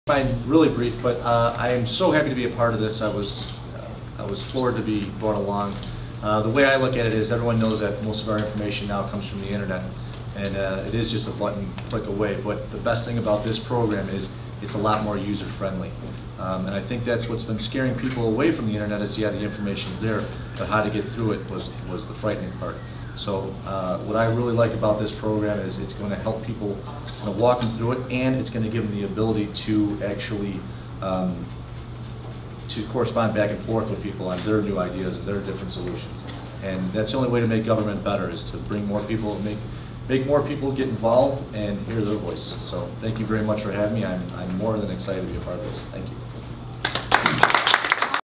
Alderman Anthony Napolitano speaks at Envision Chicago Launch
41st Ward Alderman Anthony Napolitano speaks at the launch of Envision Chicago.